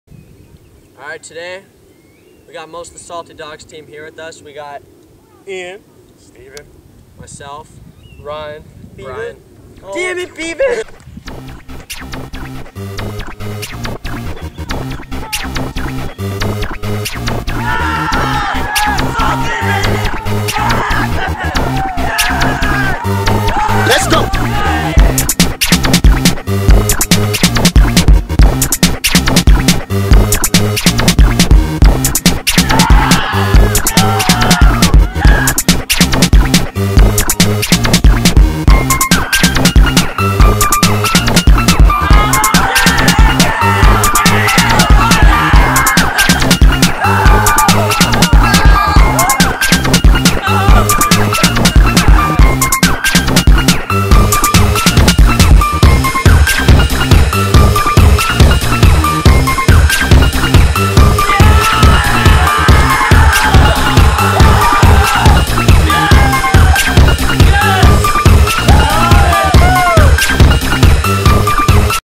Salty Dogs- Spring Break! (Amazing Trick Shots)
Shots we made over spring break! Comment and thumbs up! Stay Salty!